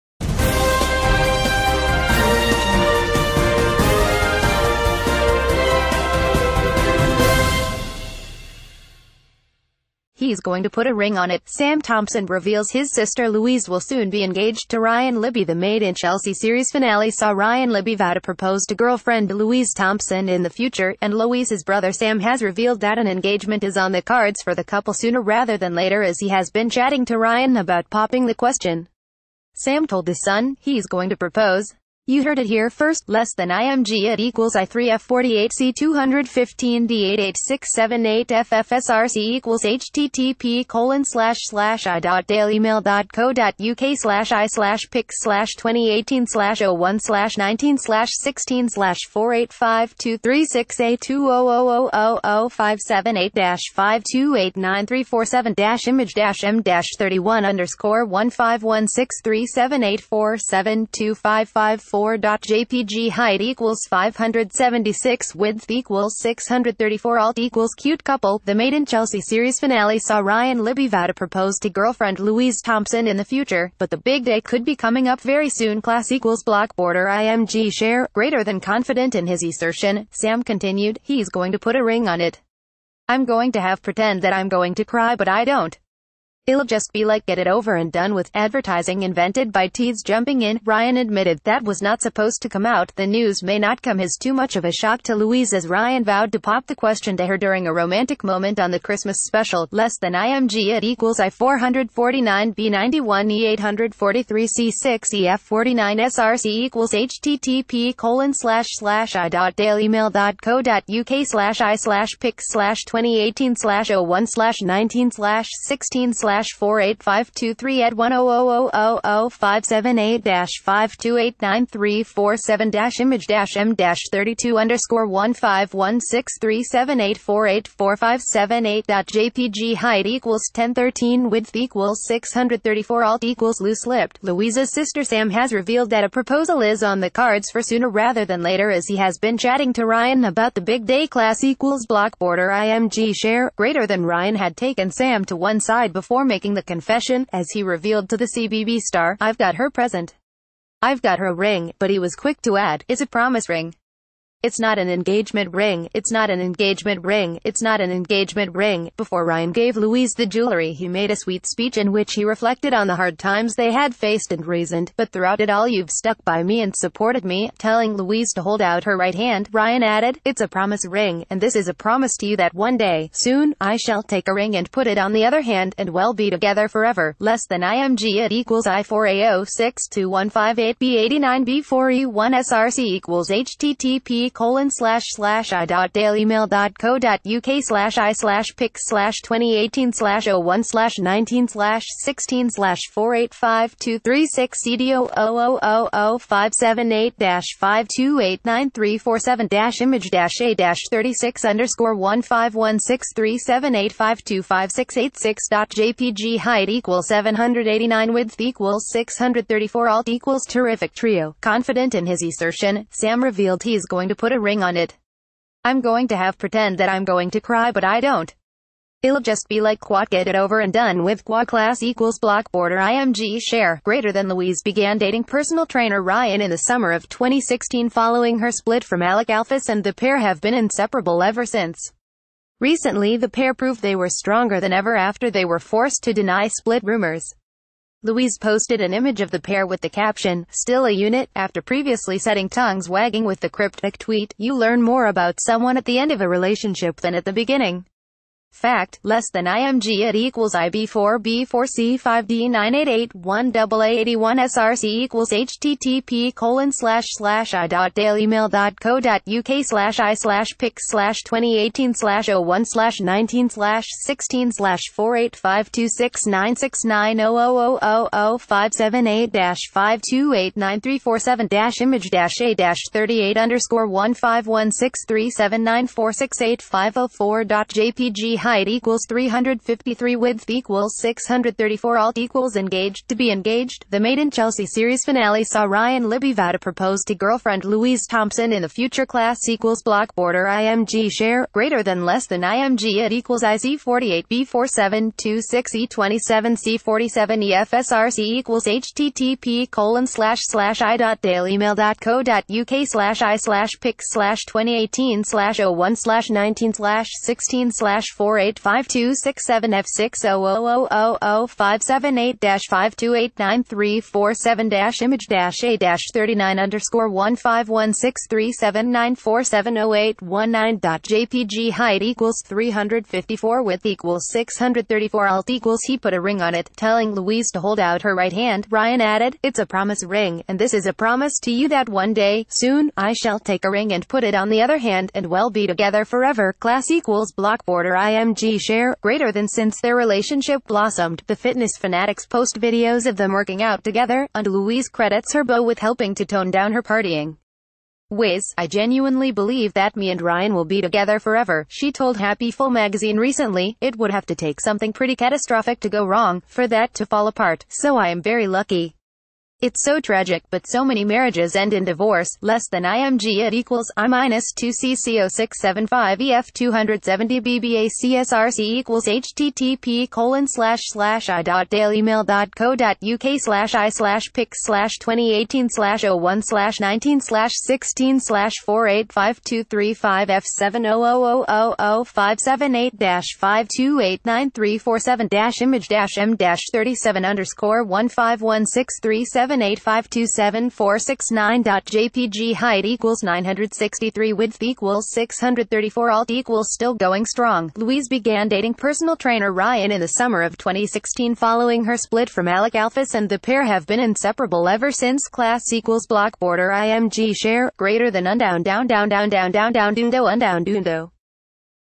' He's gonna put a ring on it ': Sam Thompson said of her sister Louise soon will be joining Ryan L
' He's gonna put a ring on it ': Sam Thompson said of her sister Louise soon will be joining Ryan Libbey
'He's going to put a ring on it': Sam Thompson reveals his sister Louise will soon be engaged to Ryan Libbey The Made In Chelsea series finale saw Ryan Libbey vow to propose to girlfriend Louise Thomp...